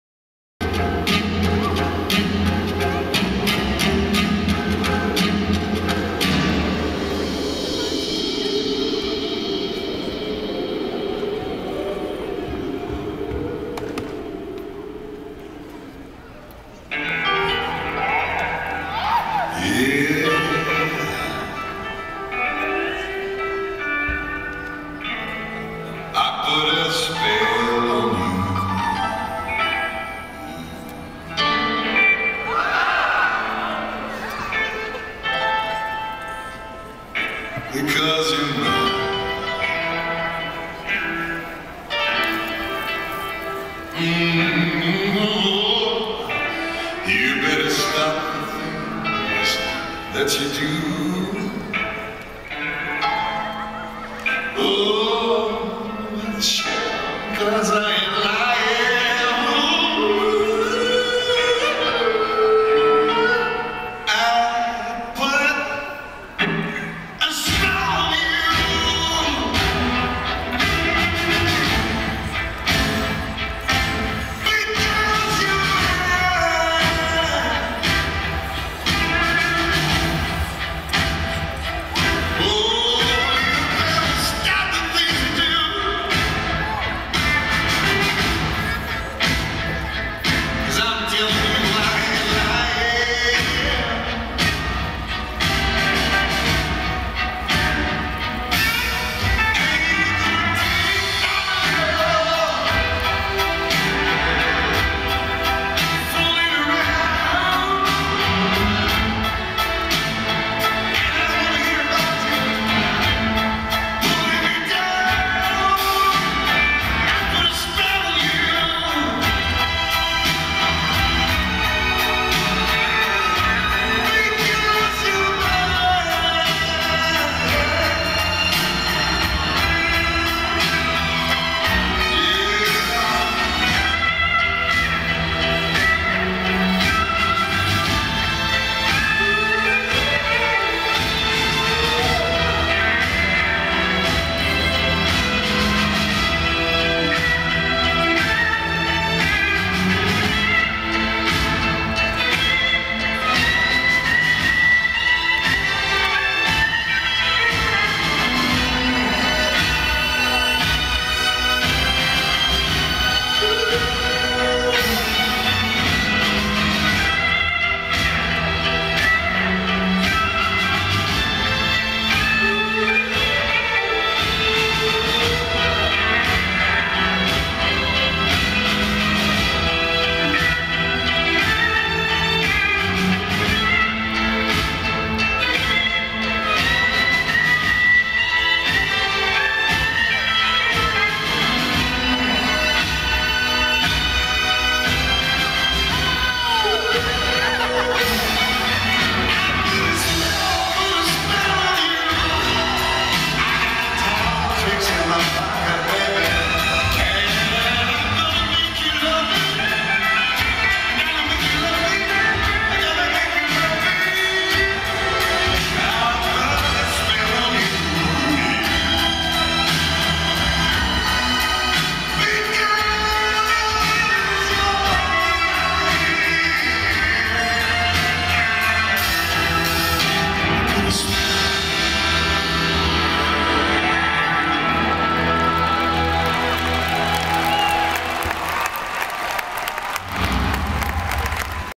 Johnny Weir in Halloween on Ice Oct 30 2015
Short Clip of Johnny Weir skating in the Halloween on Ice show Oct 30 2015 in Grand Rapids Michigan